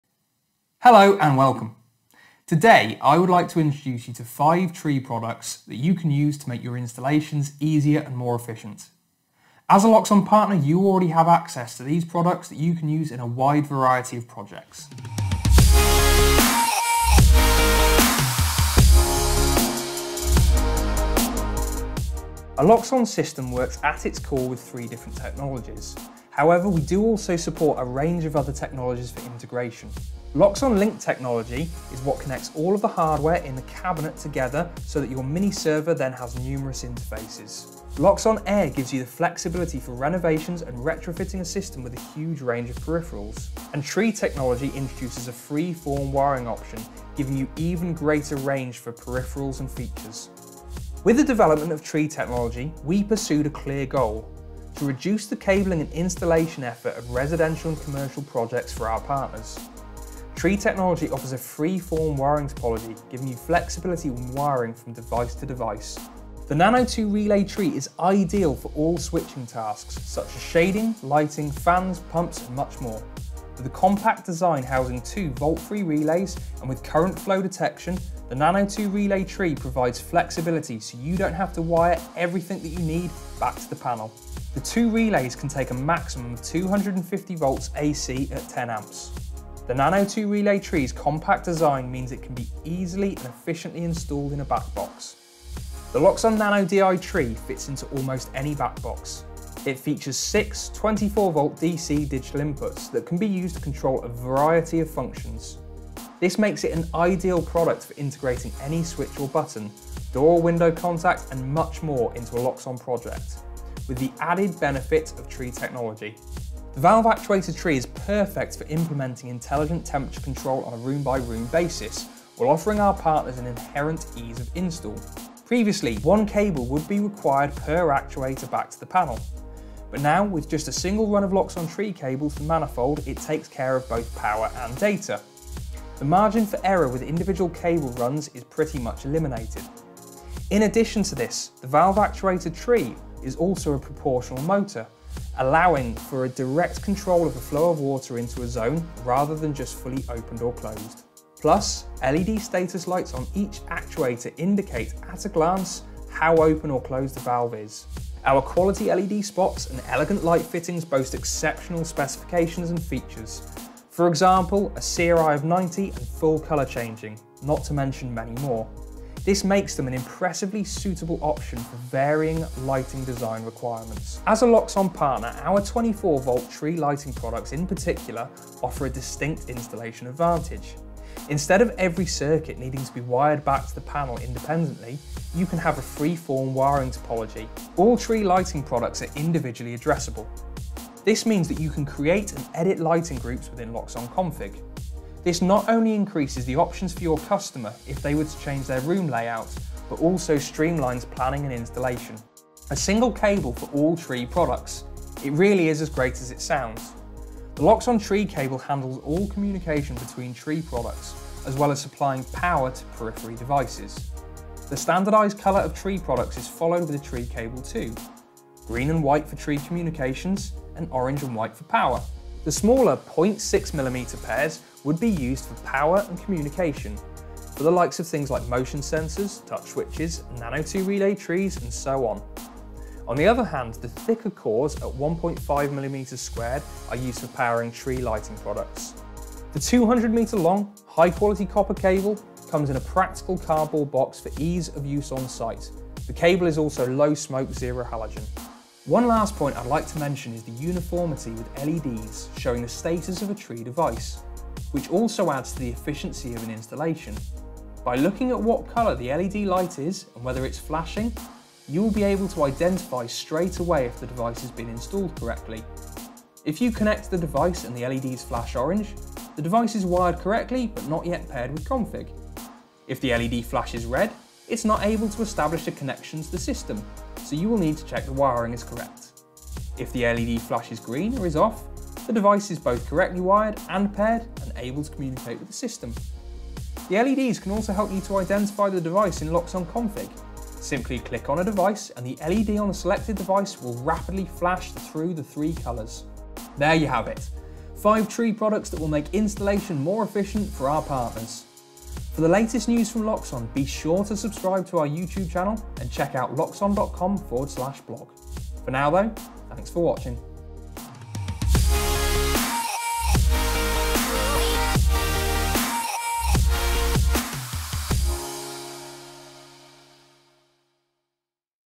In Focus: 5 Tree Products
Products in Focus: 5 Tree products for an efficient install
With the development of Tree technology, we pursued a clear goal: to reduce the cabling and installation time of a project. Coupled with the know-how of Loxone Partners, the integration of Loxone Tree devices – from LED spots to motion sensors and Touch switches – can be achieved with an unparalleled ease-of-install! Making work on an installation much more efficient.